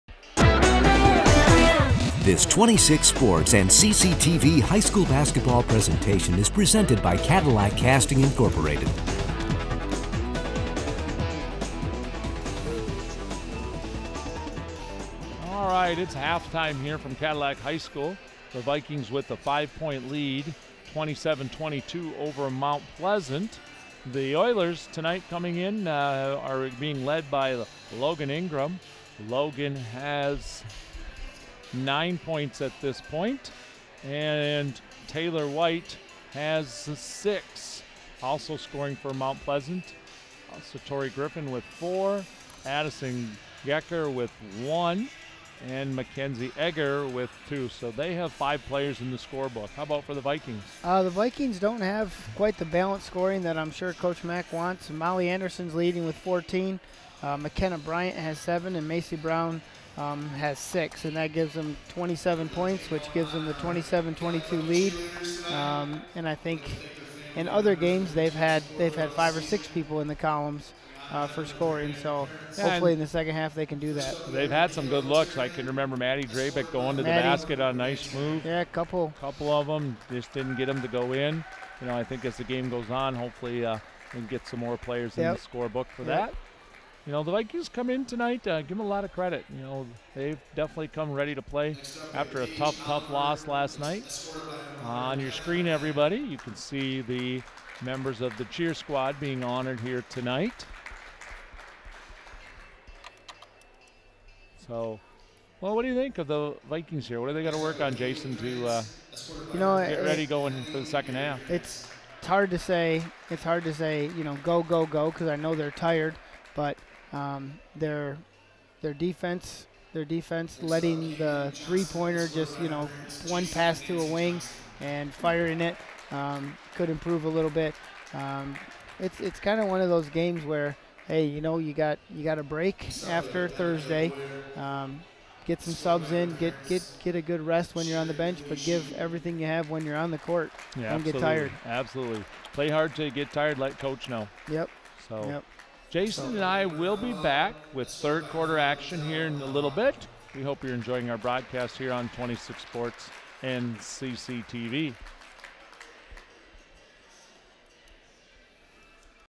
Girls Basketball Cadillac vs Mt. Pleasant 2/26/19
Watch exciting girls basketball action between Cadillac and Mt. Pleasant!